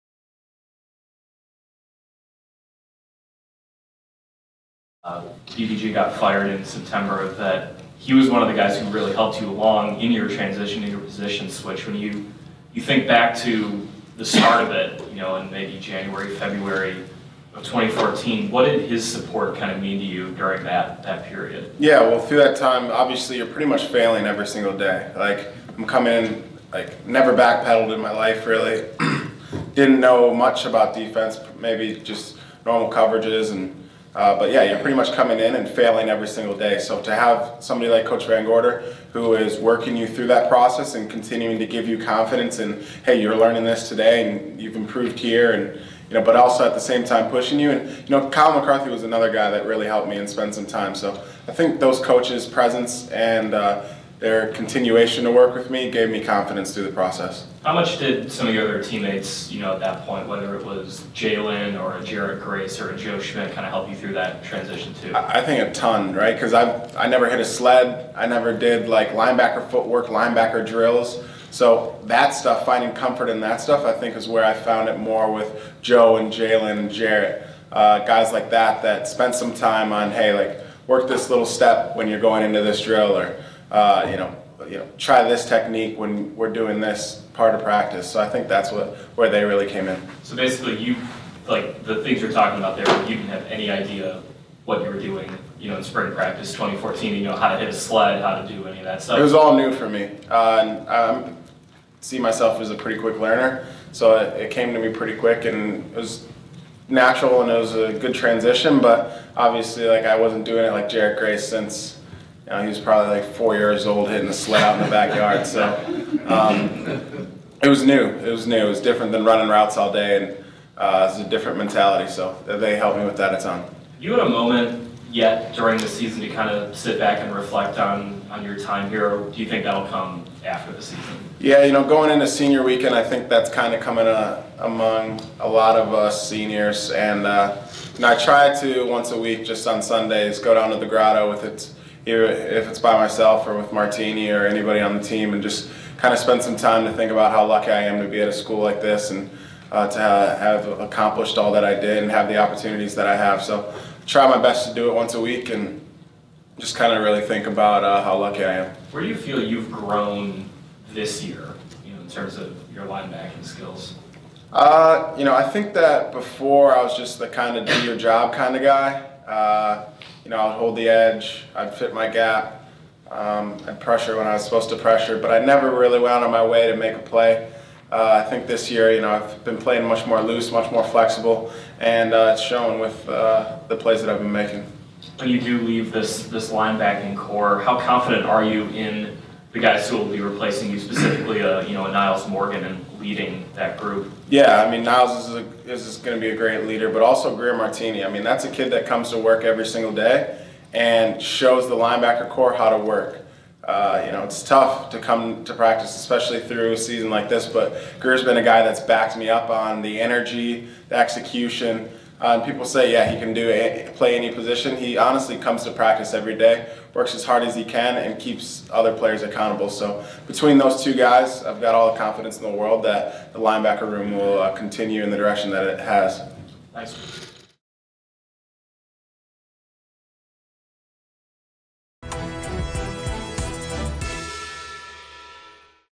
James Onwualu reflects on position switch
James Onwualu isn’t sure how his Notre Dame career would’ve turned out if not for switching from receiver to linebacker. Ahead of Senior Day, Onwualu looks back on that decision and those that helped him along the way.